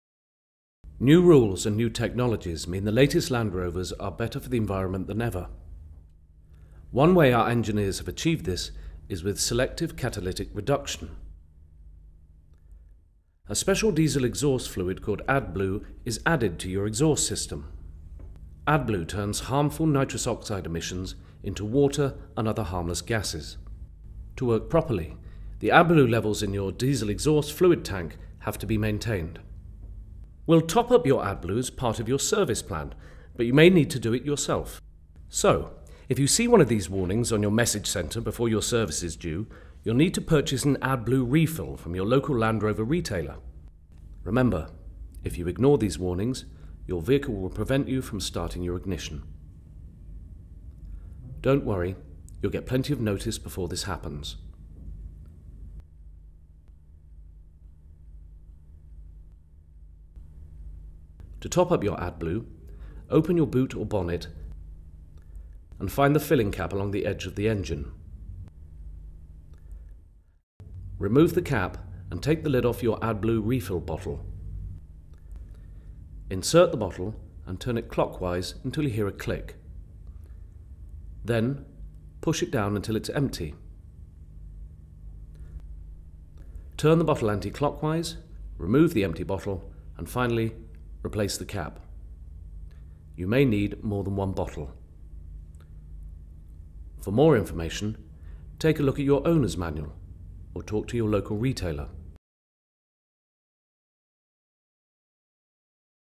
Land Rover AdBlue: What it is and how to use it
Find out everything you need to know about Land Rover AdBlue.

New rules and new technologies mean the latest Land Rovers are better for the environment than ever.

One way our engineers have achieved this, is with Selective Catalytic Reduction. A special Diesel Exhaust Fluid called AdBlue is added to your exhaust system. AdBlue turns harmful Nitrous Oxide emissions into water and other harmless gases.

To work properly, the AdBlue levels in your Diesel Exhaust Fluid tank have to be maintained.

We’ll top up your AdBlue as part of your service plan, but you may need to do it yourself, especially if your mileage is higher than average.

So, if you see one of these warnings on your message centre before your service is due, you’ll need to purchase an AdBlue refill from your local Land Rover retailer.

Remember, if you ignore these warnings, your vehicle will prevent you from starting your ignition. You’ll get plenty of notice before this happens.

To top up your AdBlue, open your bonnet and find the filling cap. Remove the cap and take the lid off your AdBlue refill bottle.

Insert the bottle and turn it clockwise until you hear a click. Then, push it down until it’s empty.

Turn the bottle anti-clockwise, remove the empty bottle and, finally, replace the cap.